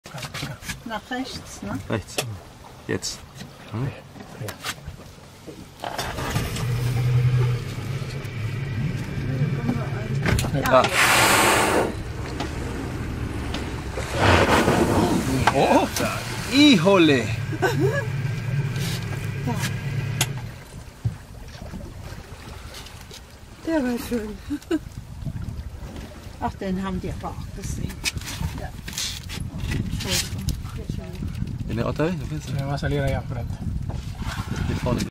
Blue Whale blowing 100 meter from the boat - Baja California - Thore Noernberg Whale Watching
Blue Whale encounter in the Loreto National Park, Baja California Mexico. Whale was seen between Isla Coronado and Isla del  Carmen